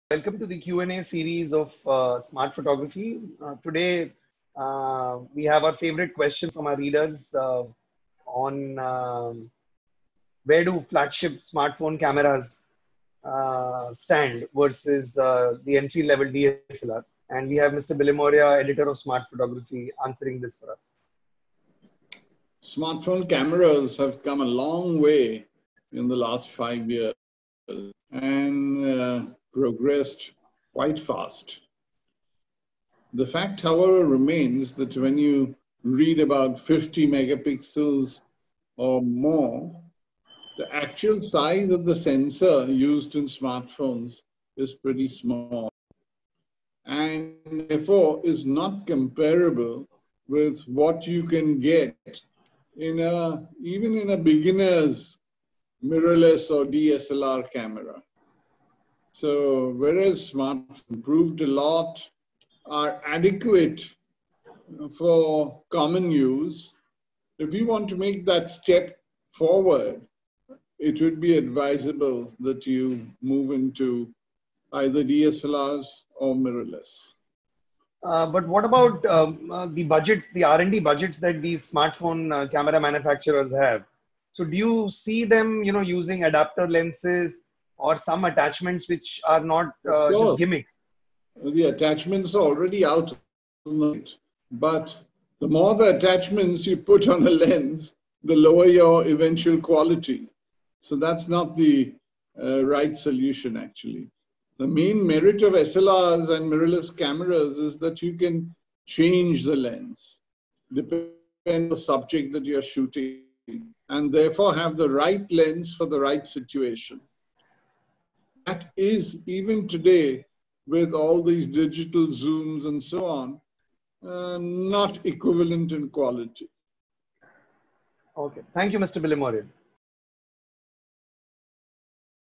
How do flagship smartphones compare with D-SLRs?
Smart Photography Editor Hoshang Billimoria gives his opinion in an answer to our readers' constant queries on Smartphone Vs D-SLRs.